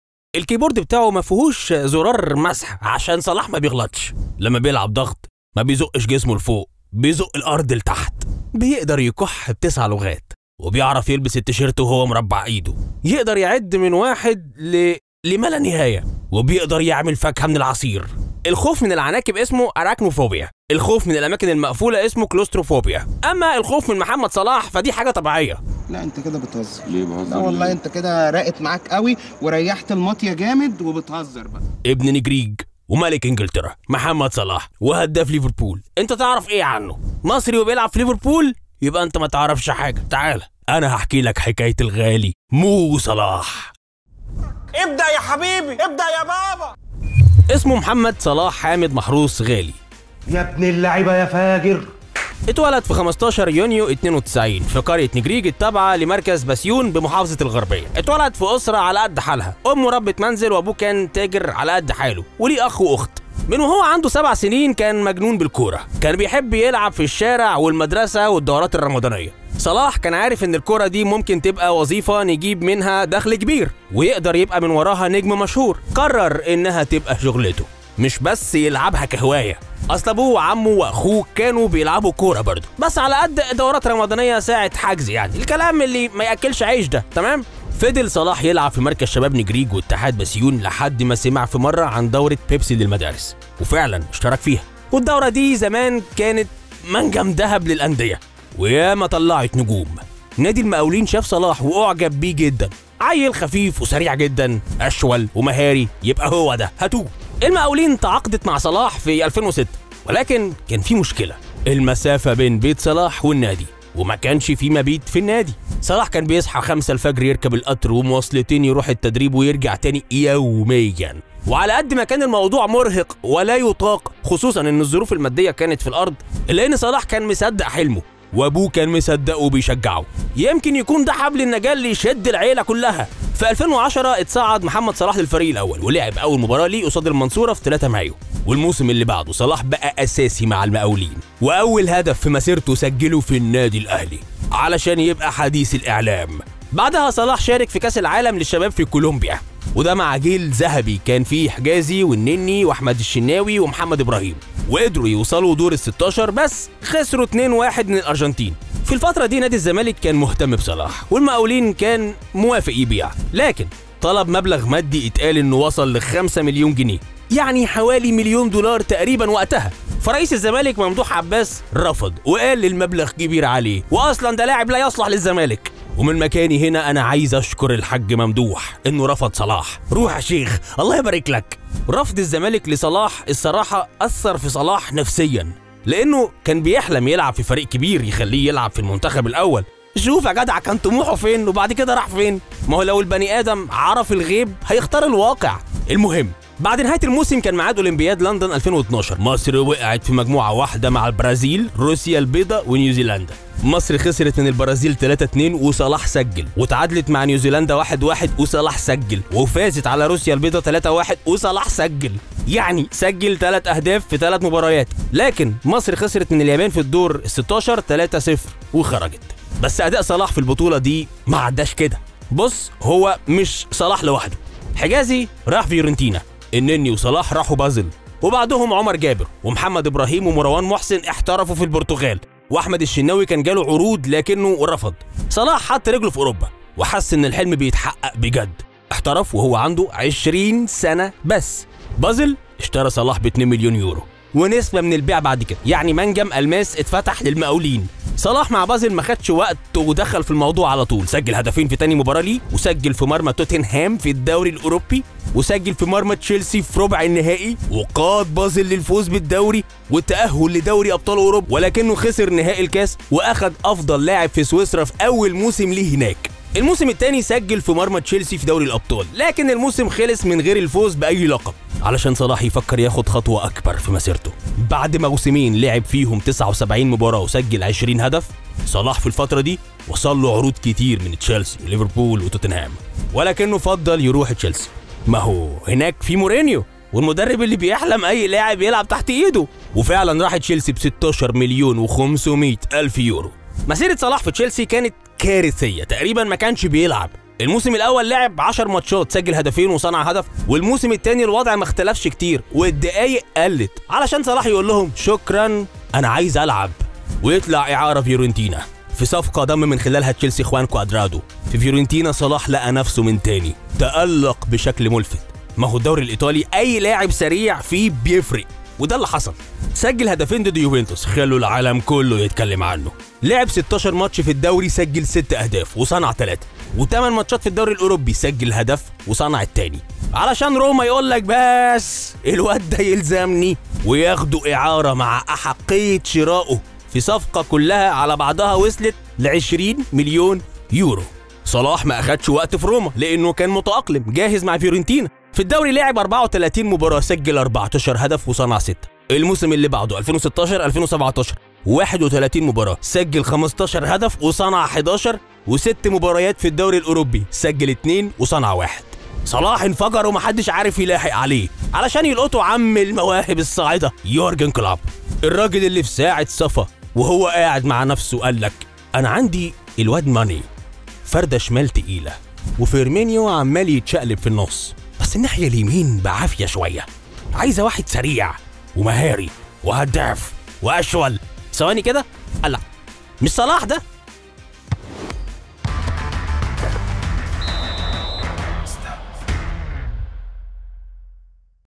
حكاية محمد_صلاح 👑 ملك مصري هز عرش إنجلترا ⚽
لاعب الصدفة، رفضه الزمالك، وانقذه ابو تريكه ..ومُباراة غيرت مصيره ..حكاية صلاح كما لم تعرفها من قبل